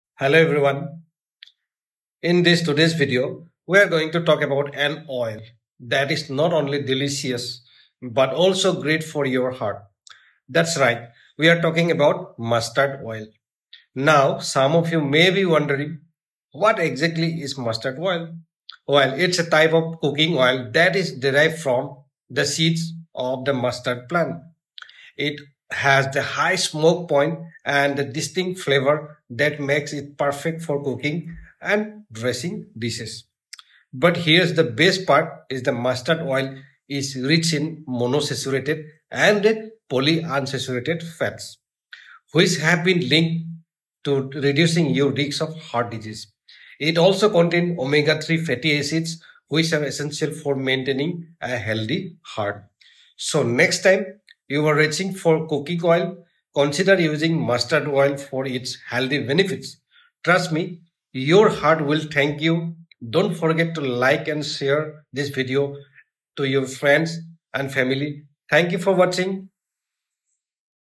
Mustard oil and its benefits for heart health
Mastered oil and its content details short and crispy video .

Hello and welcome to our video about Mustered oil.

Mustered oil is a versatile and widely used ingredient in cooking and beauty products. It is extracted from the seeds of the mustard plant, which is a member of the cabbage family.

Known for its strong pungent flavor and aroma, mustard oil is commonly used in Indian, Chinese and Southeast Asian cuisines. It adds a distinct and delicious flavor to dishes, especially curries and stir-fries.

But did you know that mustard oil also has numerous health benefits? It is rich in monounsaturated and polyunsaturated fats, which can help lower cholesterol and reduce the risk of heart disease.

In addition to its culinary uses, mustard oil is also popular in beauty care. It is often used in hair and skin treatments due to its moisturizing and anti-inflammatory properties.

So whether you're looking to spice up your meals or improve your overall health and appearance, consider adding Mustered oil to your daily routine.